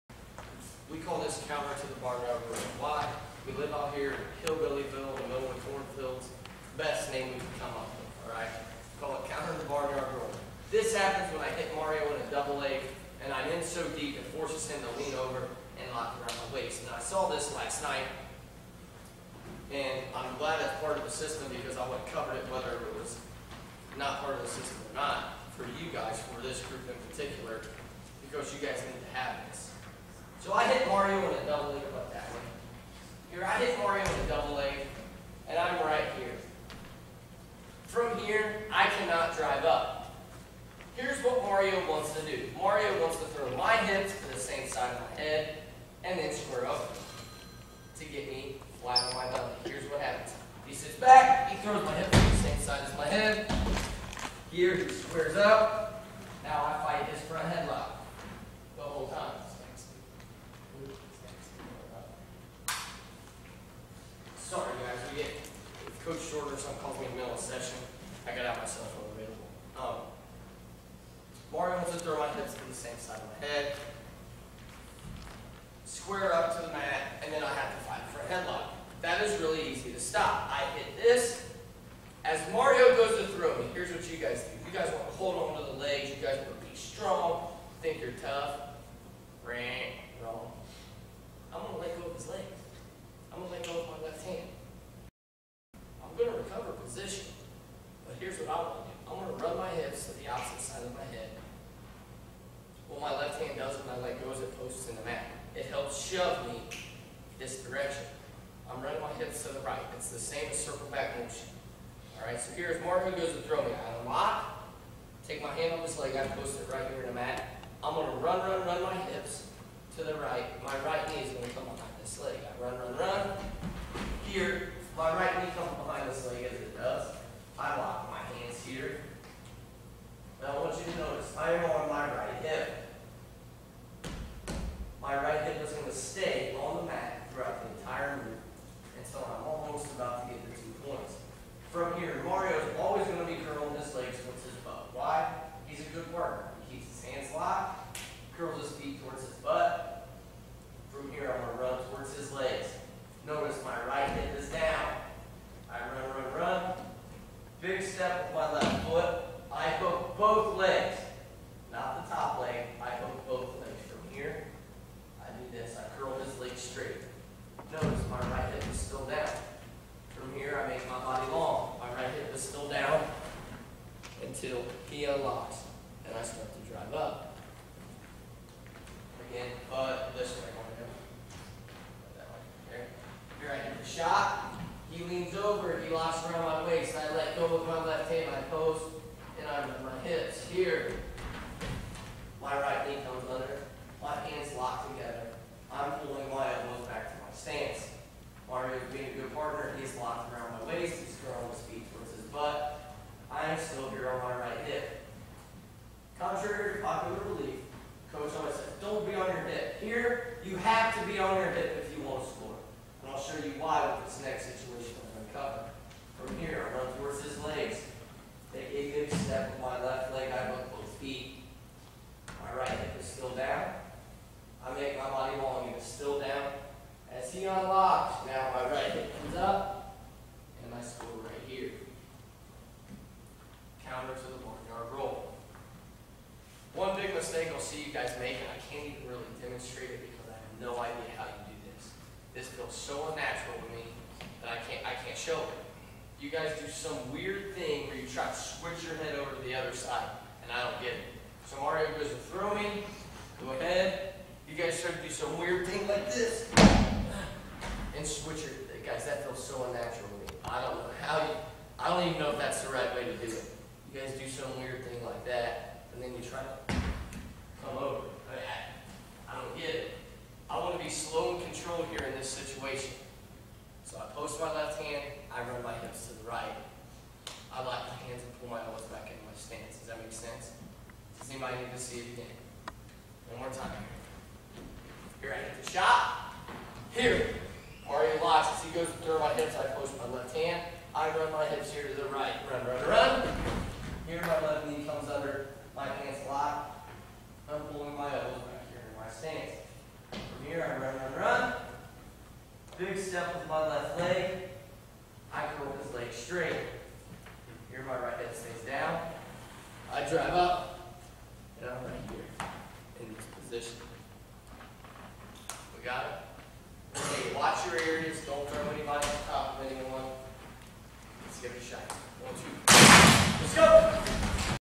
Double Leg 03 Counter To Barn Yard Roll
Use this if you get so deep on the shot the guy locks around your waist and tries to throw you through
1) As you feel the guy pulling, release left hand and post on mat
2) Run, run, run your feet to the right and put your right hip on the mat with your right knee under his bottom leg.
3) KEEP RIGHT HIP ON THE MAT
4) Wlak your feet back to the left and hook both his legs with your left foot
5) Drive in and then bring your right hip off the mat.